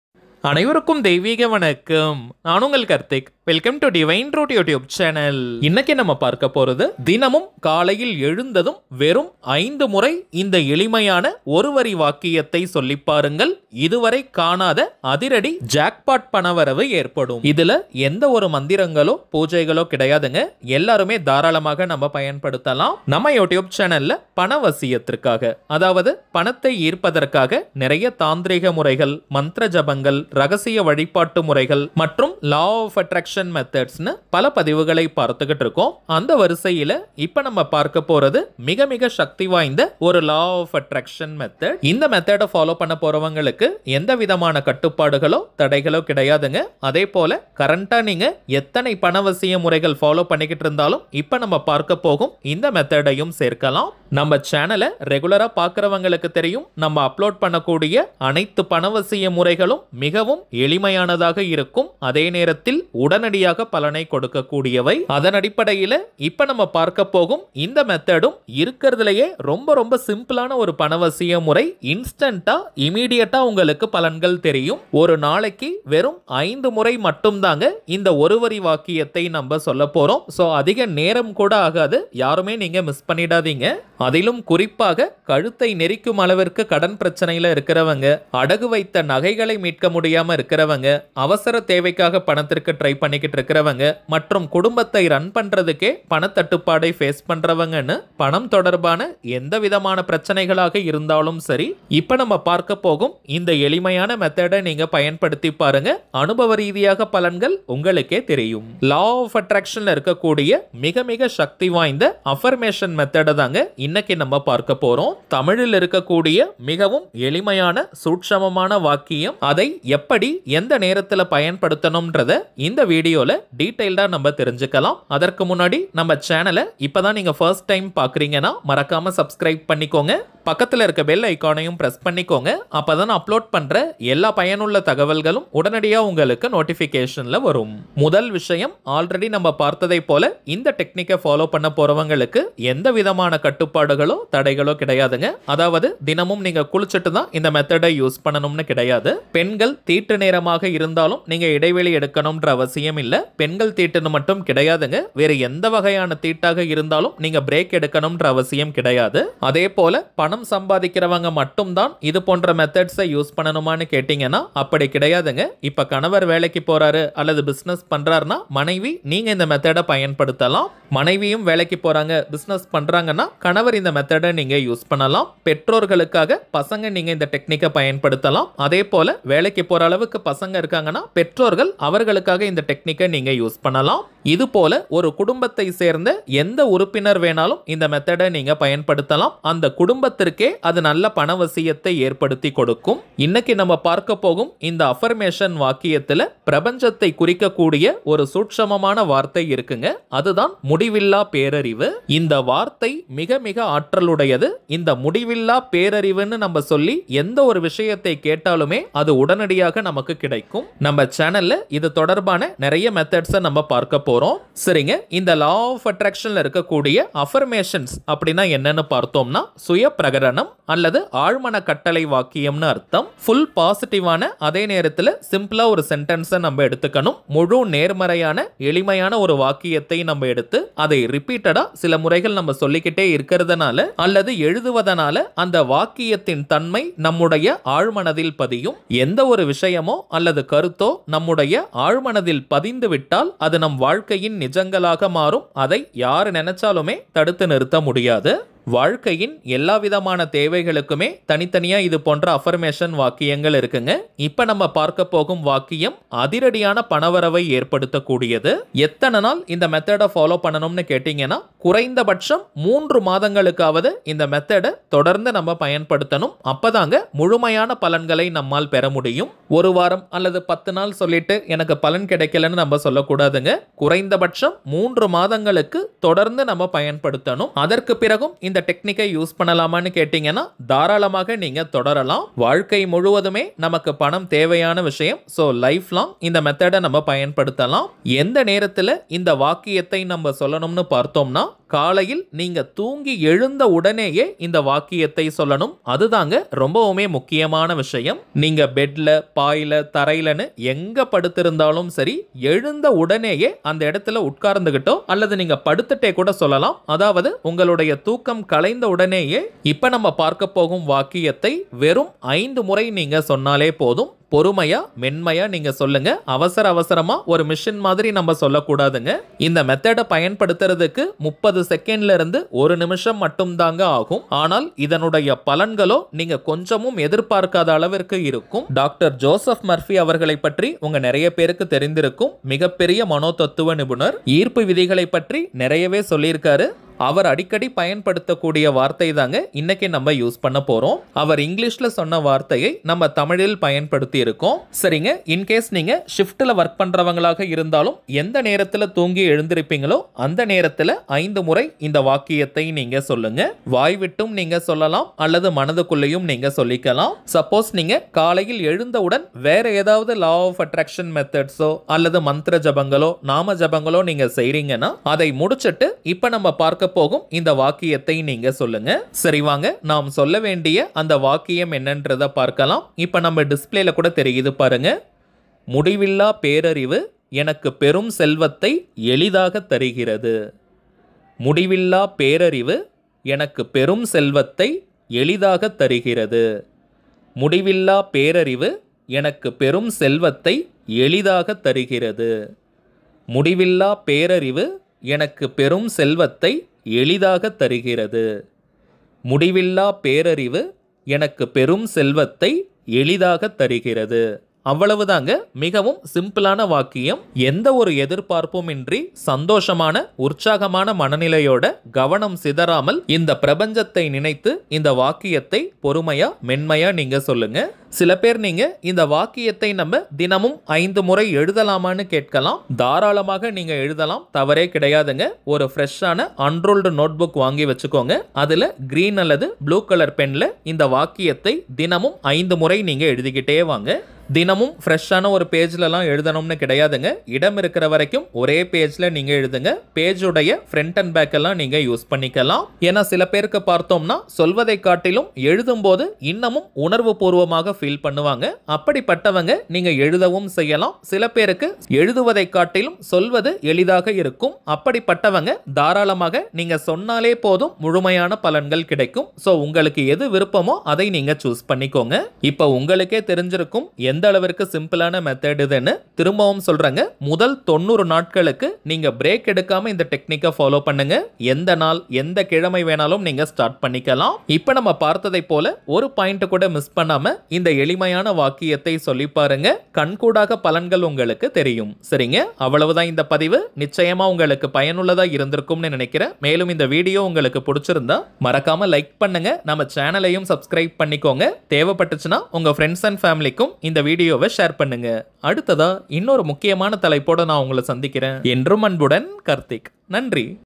காலையில் எழுந்ததும் வெறும் 5 முறை இந்த 1 வரியை சொல்லி பாருங்க அதிரடி ஜாக்பாட் பணவரவு ஏற்படும்
For Bussiness Promotion and Collaboration

Hi Everyone !! Welcome to Divine route youtube channel !! In this video I have explained simple money attraction technique. How to attract money is explained in this video. To know more watch this video..Enjoy the video...